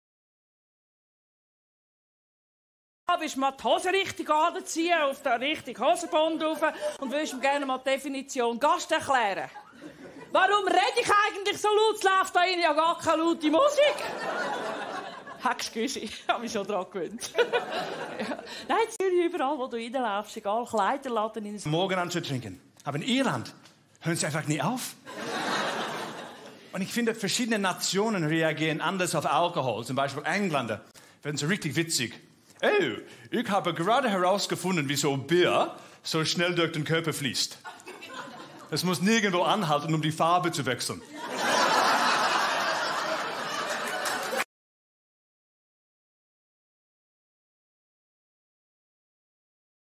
Da bleibt kein Auge trocken! Helga Schneider & Rob Spence Comedy Night
Die Comedy-Legenden Helga Schneider und Rob Spence stehen an einem Abend nur in Andermatt auf der Bühne! Verpassen Sie nicht Ihre Gelegenheit, am 7. August 2021 die Nacht durchzulachen. Die Tickets sind begrenzt – buchen Sie noch heute